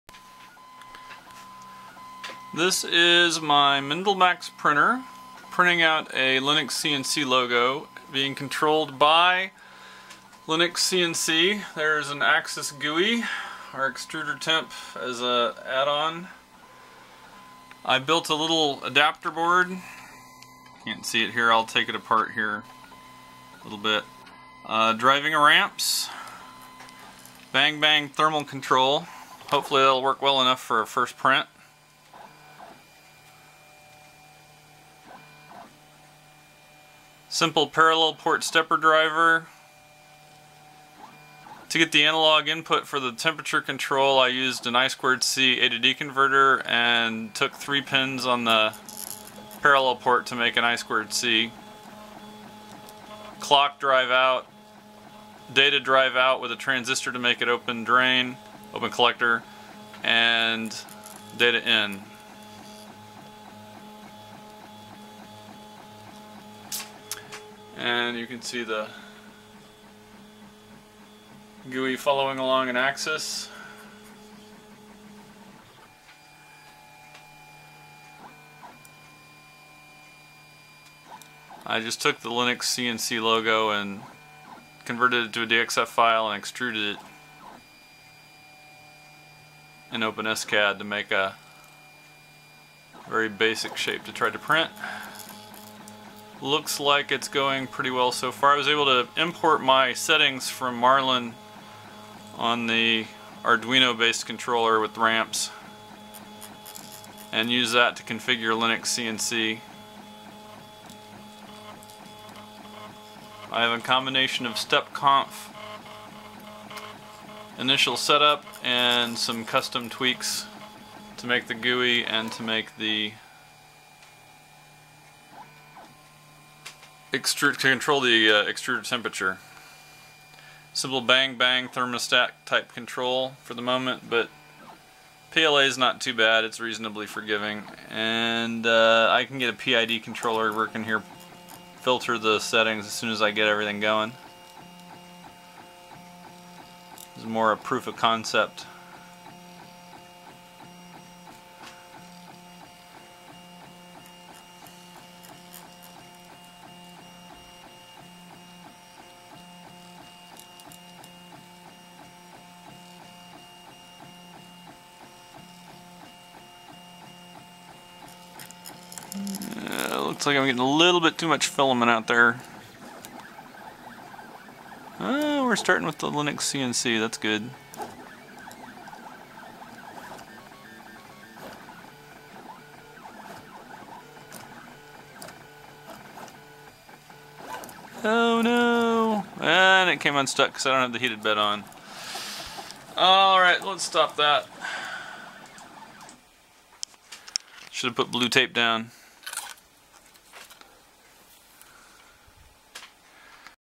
LinuxCNC & MendelMax
Controlling my MendelMax with LinuxCNC

HAL code and details of the interface board can be found on github